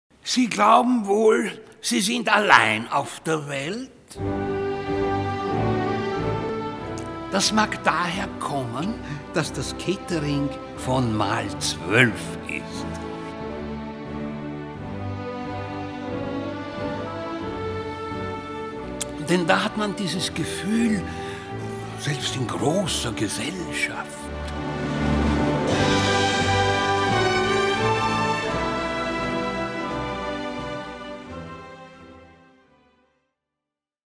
Mahl 12 – Kinospot 03
Kinspot zur Mahl 12 Kampagne "Einzigartig. Selbst in großer Gesellschaft".